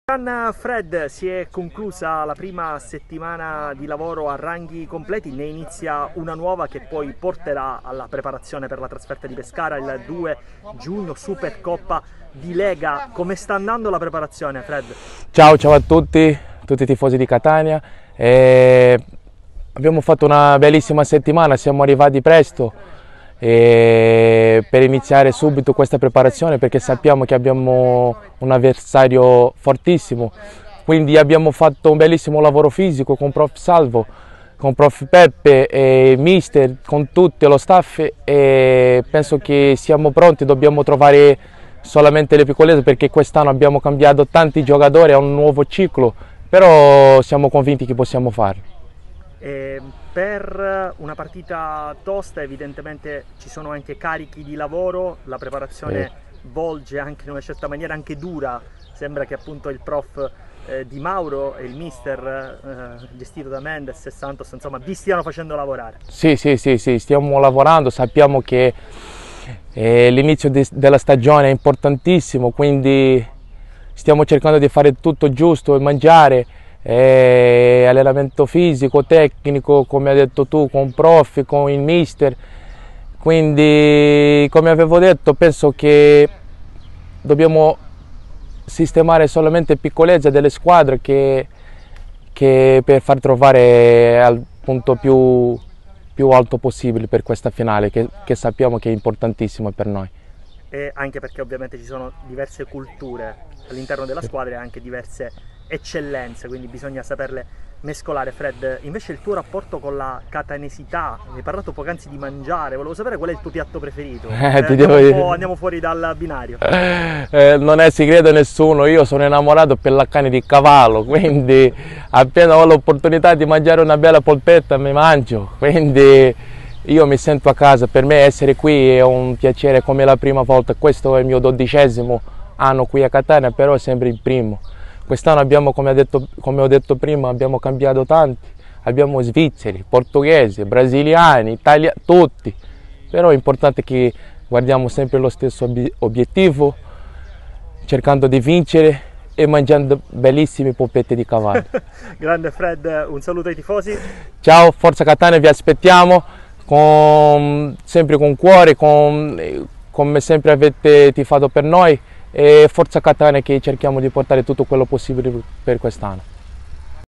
Fred Cabral: "Siamo pronti per la Supercoppa"
Le dichiarazioni del Capitano, Fred Cabral, in vista della Supercoppa Italiana 2022.